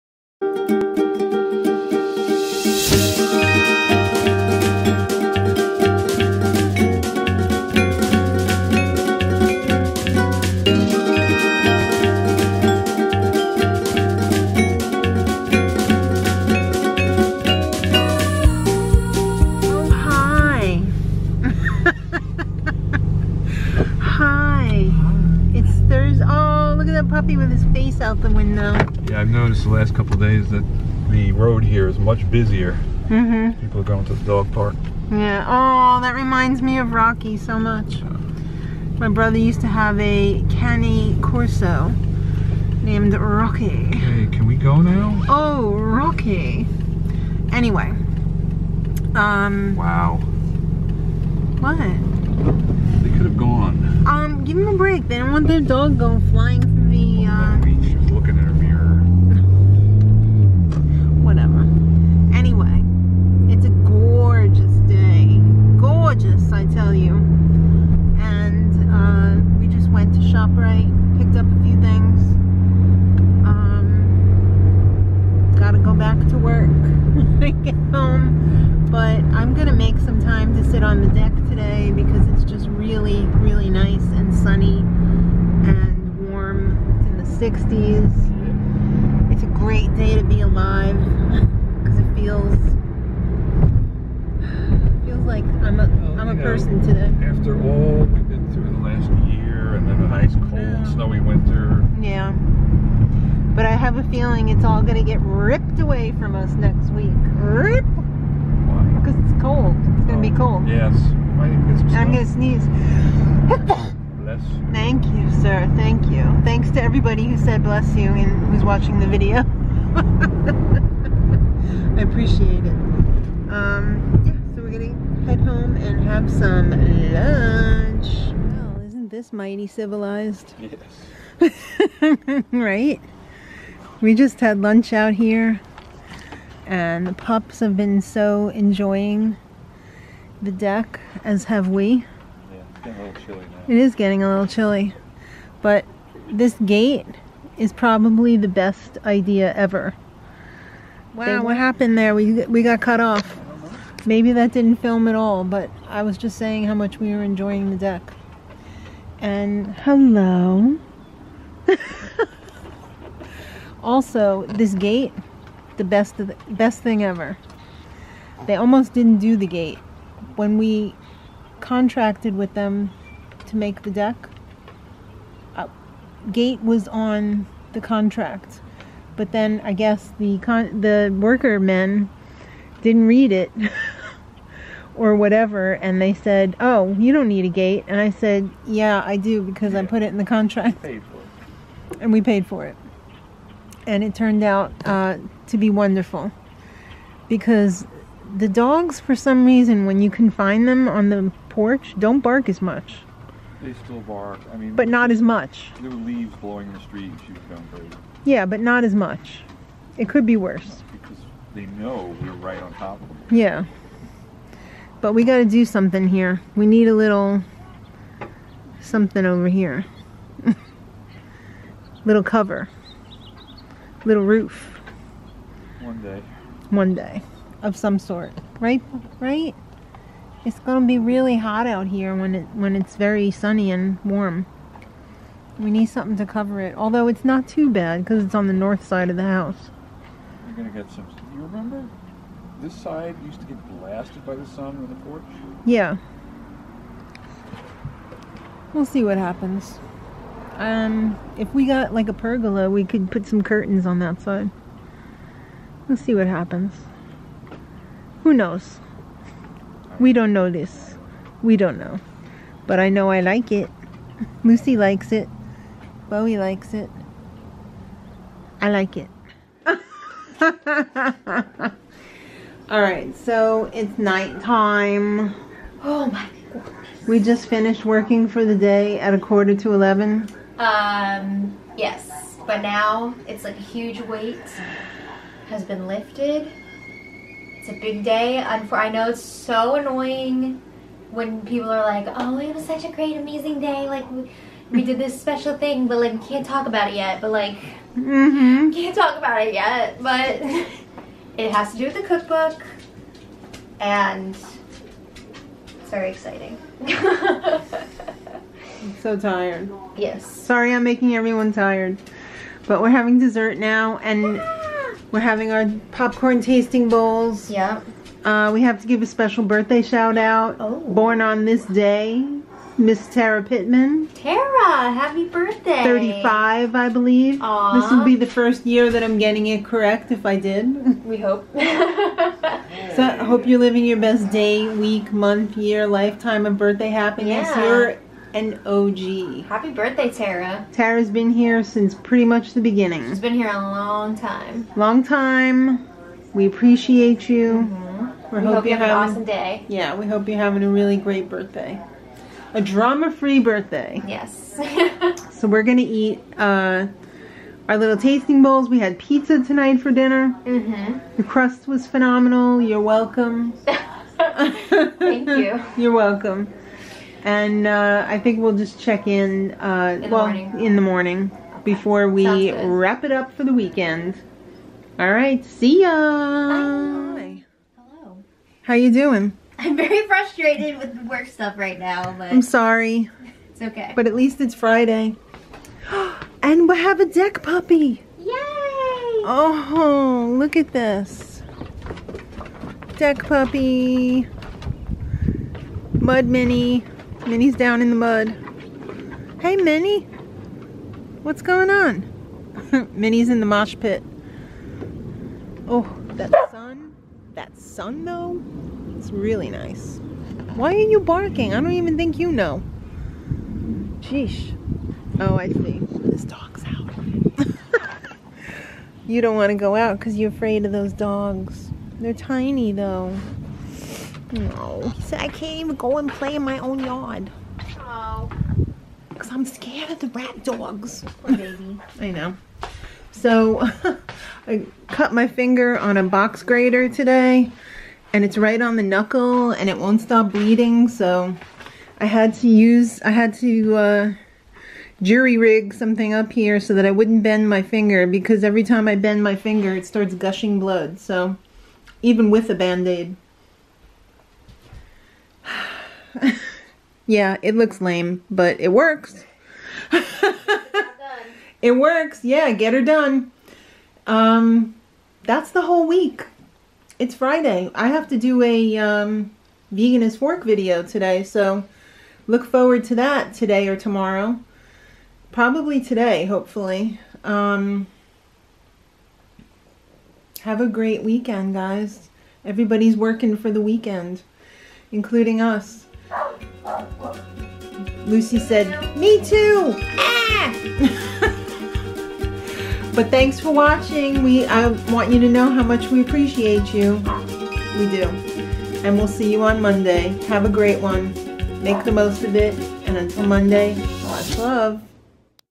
Time to Enjoy the Deck!
Time to Enjoy the Deck! (03/11/21)
We made it! That's the whole week - the deck was pretty amazing today - worth every penny! Thanks for being awesome - we much love you!

ON THIS DAY:
1 Year Ago:
2 Years Ago:
3 Years Ago:
4 Years Ago: